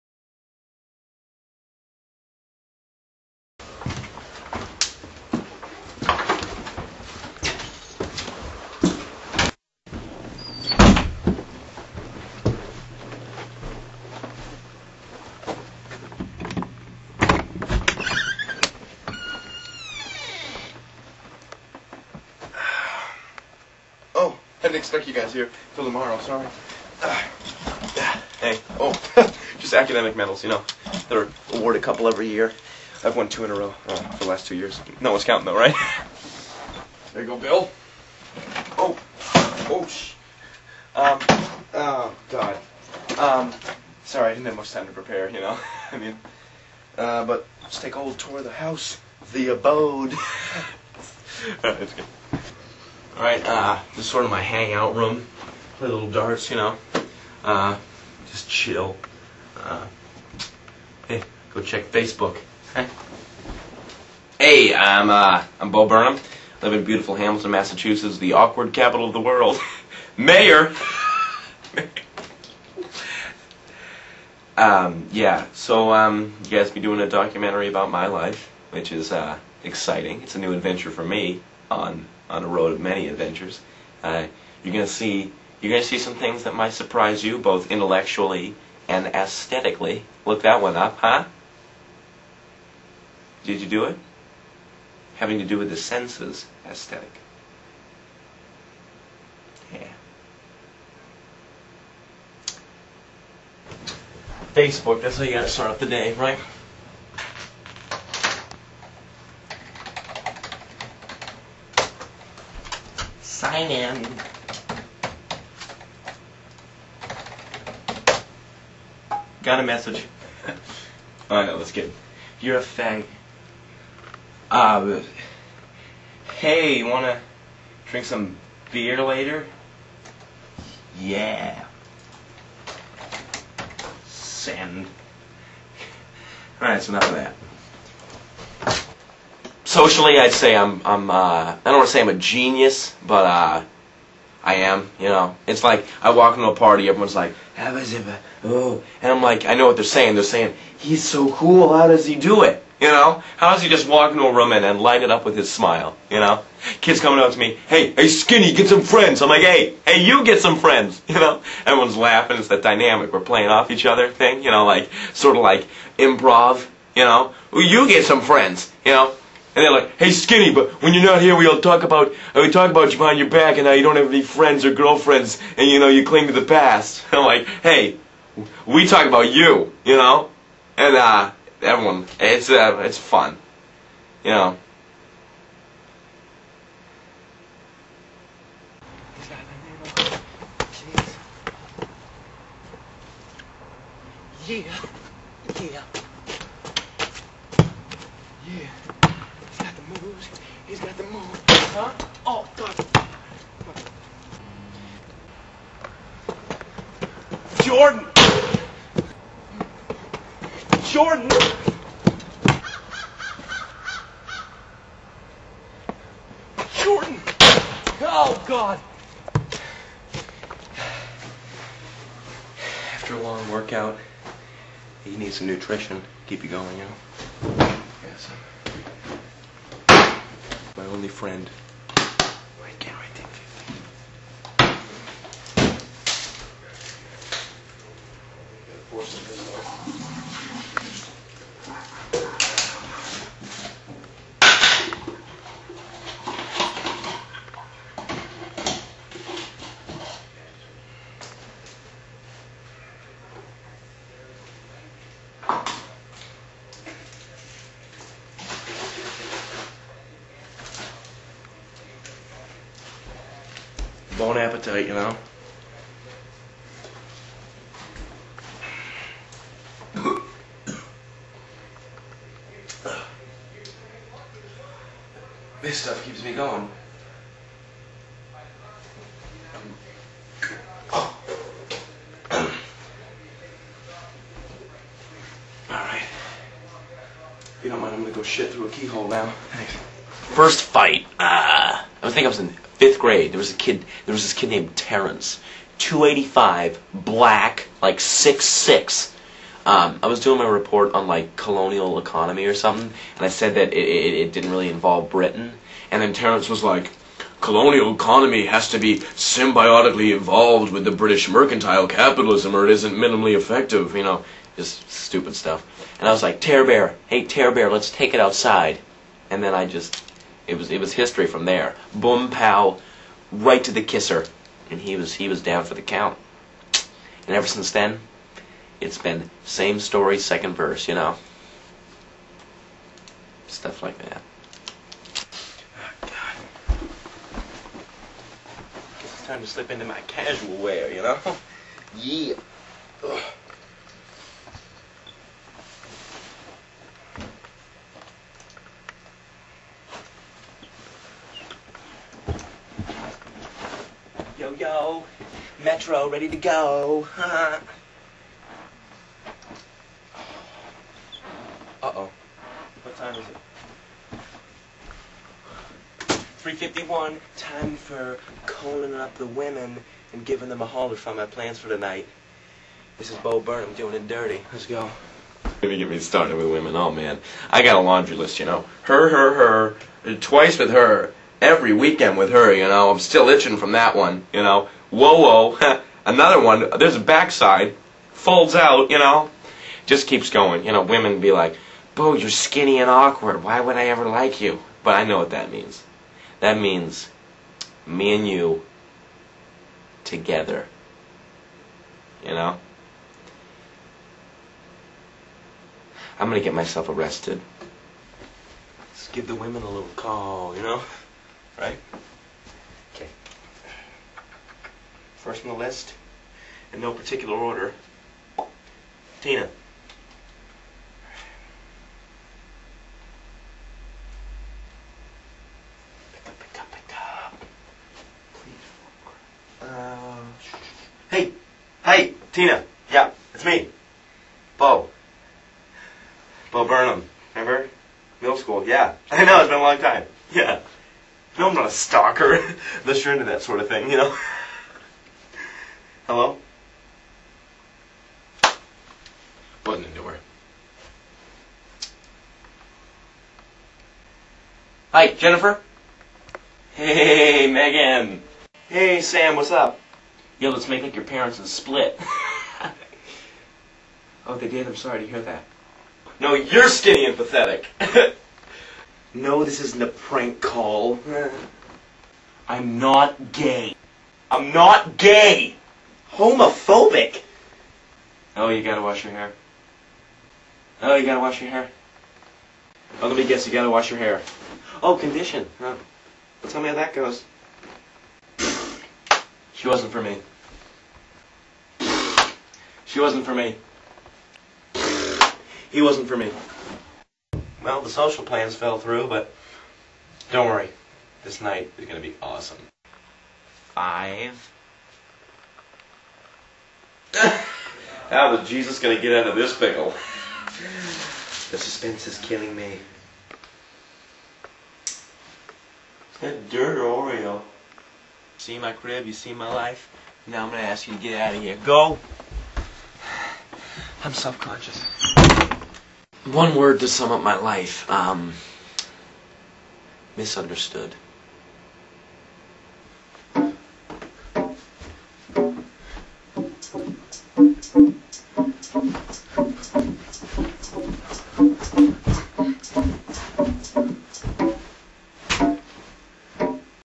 A Day in the Life
a VERY old video
i made this probably three years ago.
since i havent posted anything in a while i thought i'd post this old video.
i will be posting new things soon.
this is sort of an "office"/guest movie inspired thing.

-bo burnham